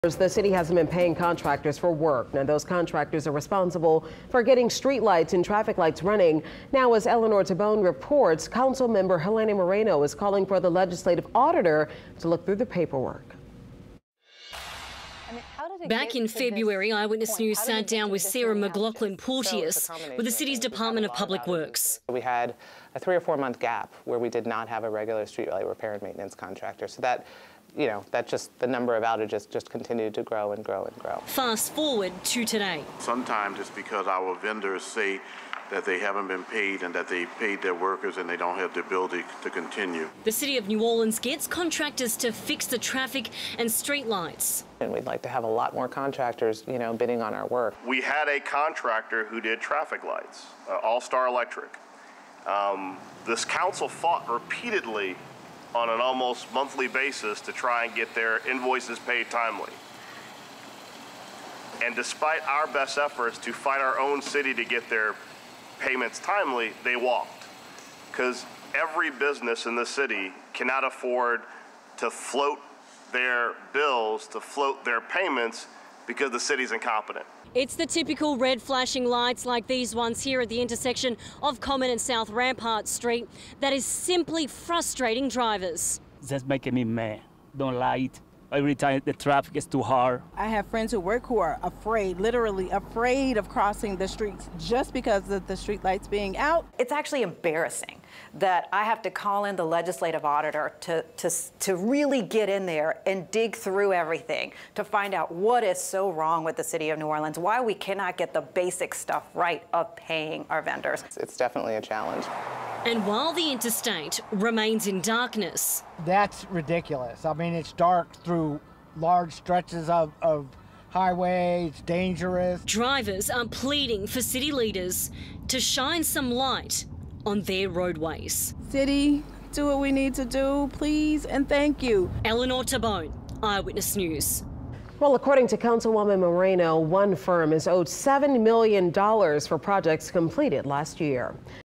"The city is incompetent" - City Council says contractors aren't being paid for work
Those contractors are the ones that fix the street and traffic lights, without them the city's roads will remain in darkness.